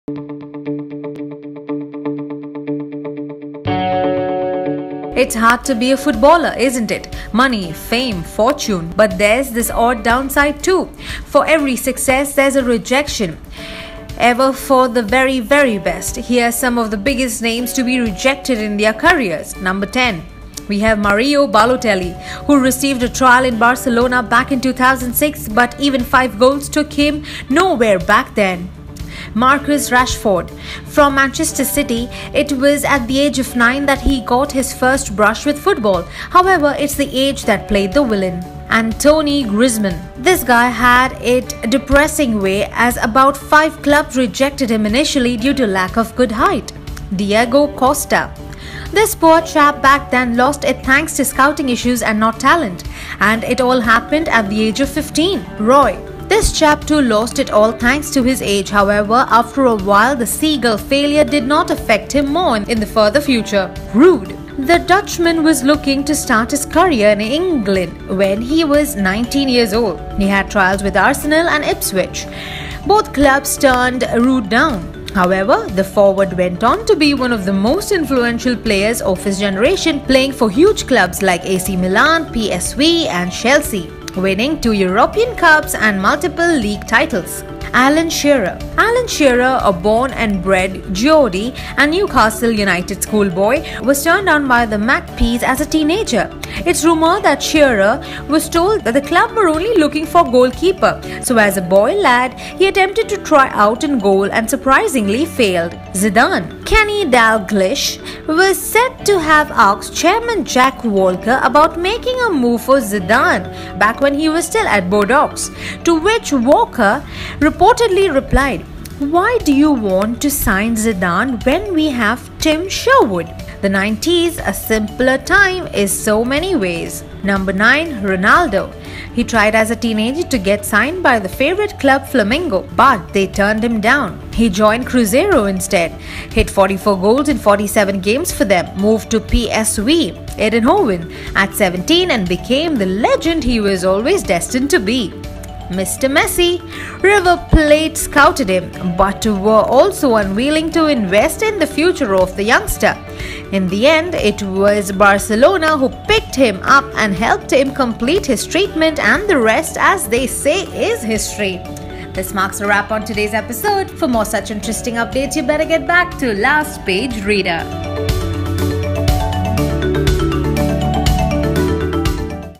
Top 10 Footballers Who Were Rejected As Youngsters | Last Page Readers
It's hard to be a footballer, isn't it? Money, fame, fortune... but there's the odd downside, too. For every success, there's a rejection - even for the very, very best. Here's some of the biggest names to be rejected in their careers.